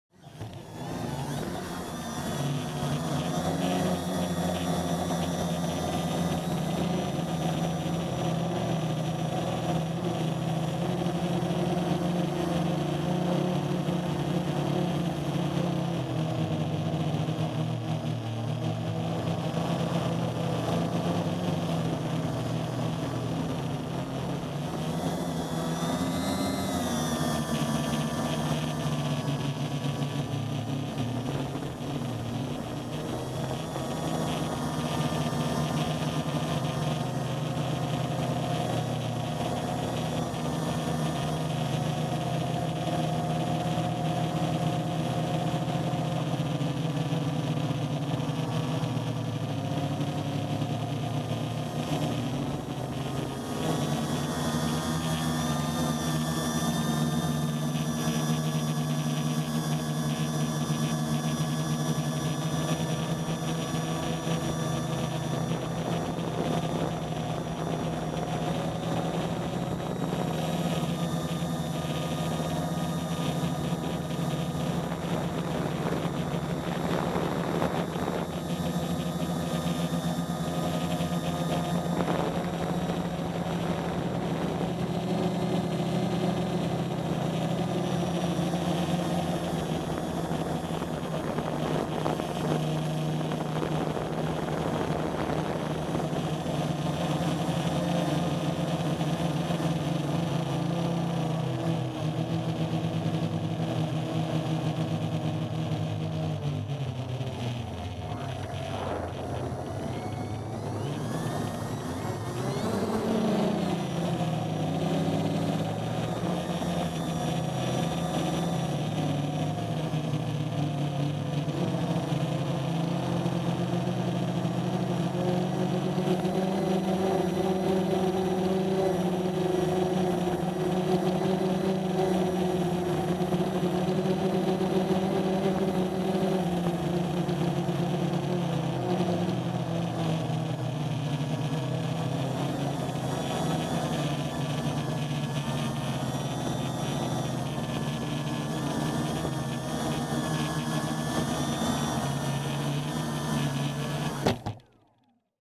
Checking the Highland Cattle with a Drone
Quick run out around the paddock in between rain showers.
1000kv motors 10/4.5 props with KK2 Board